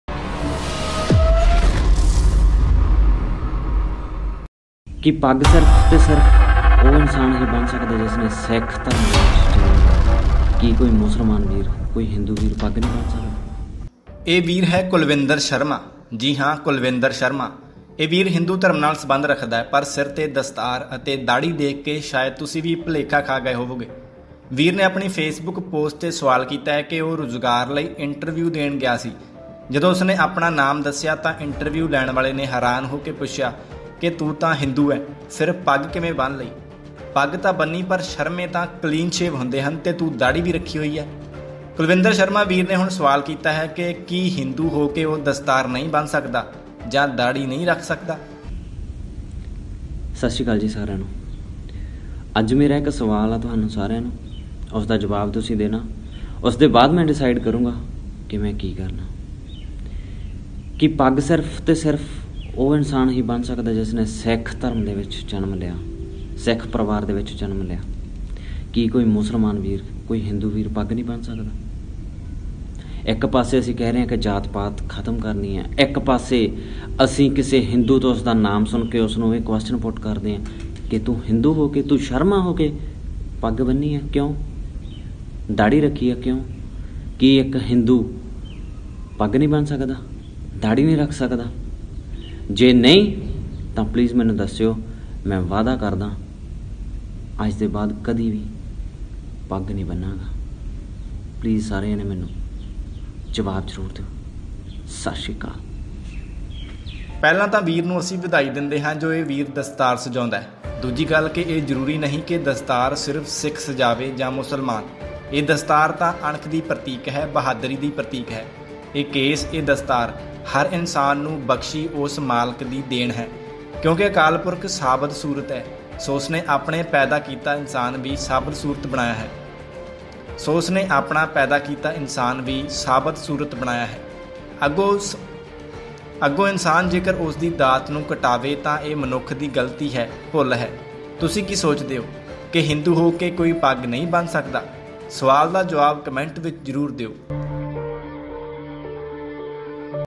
ਕੀ ਕੋਈ ਹਿੰਦੂ ਦਸਤਾਰ ਨਹੀਂ ਬੰਨ ਸਕਦਾ ?? ਇਸ ਨੌਜਵਾਨ ਨੇ ਕੀਤਾ ਸਵਾਲ
ਇਹ ਵੀਰ ਹੈ ਕੁਲਵਿੰਦਰ ਸ਼ਰਮਾ,ਜੀ ਹਾਂ ਕੁਲਵਿੰਦਰ ਸ਼ਰਮਾ। ਇਹ ਵੀਰ ਹਿੰਦੂ ਧਰਮ ਨਾਲ ਸਬੰਧ ਰੱਖਦਾ ਹੈ ਪਰ ਸਿਰ ਤੇ ਦਸਤਾਰ ਤੇ ਦਾਹੜੀ ਦੇਖਕੇ ਸ਼ਾਇਦ ਤੁਸੀਂ ਵੀ ਭੁਲੇਖਾ ਖਾ ਗਏ ਹੋਵੋਗੇ। ਵੀਰ ਨੇ ਆਪਣੀ ਫੇਸਬੁੱਕ ਪੋਸਟ ਤੇ ਸਵਾਲ ਕੀਤਾ ਹੈ ਕਿ ਉਹ ਰੁਜਗਾਰ ਲਈ ਇੰਟਰਵਿਊ ਦੇਣ ਗਿਆ ਸੀ। ਜਦੋਂ ਉਸਨੇ ਆਪਣਾ ਨਾਮ ਦੱਸਿਆ ਤਾਂ ਇੰਟਰਵਿਊ ਲੈਣ ਵਾਲੇ ਨੇ ਹੈਰਾਨ ਹੋ ਕੇ ਪੁੱਛਿਆ ਕਿ ਤੂੰ ਤਾਂ ਹਿੰਦੂ ਹੈਂ ਫਿਰ ਪੱਗ ਕਿਵੇਂ ਬੰਨ ਲਈ ?? ਪੱਗ ਤਾਂ ਬੰਨੀ,ਪਰ ਸ਼ਰਮੇ ਤਾਂ ਕਲੀਨ ਸ਼ਿਵ ਹੁੰਦੇ ਹਨ ਤੇ ਤੂੰ ਤਾਂ ਦਾਹੜੀ ਵੀ ਰੱਖੀ ਹੋਈ ??  ਕੁਲਵਿੰਦਰ ਸ਼ਰਮਾ ਵੀਰ ਨੇ ਹੁਣ ਸਵਾਲ ਕੀਤਾ ਹੈ ਕਿ ਕੀ ਹਿੰਦੂ ਹੋ ਕੇ ਉਹ ਦਸਤਾਰ ਨਹੀਂ ਬੰਨ ਸਕਦਾ ? ਜਾਂ ਦਾਹੜੀ ਨਹੀਂ ਰੱਖ ਸਕਦਾ ??

ਪਹਿਲਾਂ ਤਾਂ ਵੀਰ ਨੂੰ ਅਸੀਂ ਵਧਾਈ ਦਿੰਨੇ ਹਾਂ ਜੋ ਇਹ ਵੀਰ ਦਸਤਾਰ ਸਜਾਉਂਦਾ ਹੈ। ਦੂਜੀ ਗੱਲ ਕਿ ਇਹ ਜਰੂਰੀ ਨਹੀਂ ਕਿ ਦਸਤਾਰ ਸਿਰਫ ਸਿੱਖ ਸਜਾਵੇ ਜਾਂ ਮੁਸਲਮਾਨ,ਇਹ ਦਸਤਾਰ ਤਾਂ ਅਣਖ ਦੀ ਪ੍ਰਤੀਕ ਹੈ,ਬਹਾਦਰੀ ਦਾ ਪ੍ਰਤੀਕ ਹੈ। ਇਹ ਕੇਸ,ਇਹ ਦਸਤਾਰ ਹਰ ਇਨਸਾਨ ਨੂੰ ਬਖਸੀ ਉਸ ਮਾਲਕ ਦੀ ਦੇਣ ਹੈ। ਕਿਉਂਕਿ ਅਕਾਲ ਪੁਰਖ ਸਾਬਤ ਸੂਰਤ ਹੈ ਸੋ ਉਸਨੇ ਆਪਣਾ ਪੈਦਾ ਕੀਤਾ ਮਨੁੱਖ ਵੀ ਸਾਬਤ ਸੂਰਤ ਬਣਾਇਆ ਹੈ। ਅੱਗੋਂ ਮਨੁੱਖ ਜੇਕਰ ਉਸਦੀ ਦਾਤ ਨੂੰ ਕਟਾਵੇ ਤਾਂ ਇਹ ਮਨੁੱਖ ਦੀ ਭੁੱਲ ਤੇ ਗਲਤੀ ਹੈ। ਤੁਸੀਂ ਕੀ ਸੋਚਦੇ ਹੋ ? ਕੀ ਹਿੰਦੂ ਹੋ ਕੇ ਕੋਈ ਪੱਗ ਨਹੀਂ ਬੰਨ ਸਕਦਾ ?? ਸਵਾਲ ਦਾ ਜਵਾਬ ਕਮੈਂਟ ਵਿਚ ਜਰੂਰ ਦਿਓ।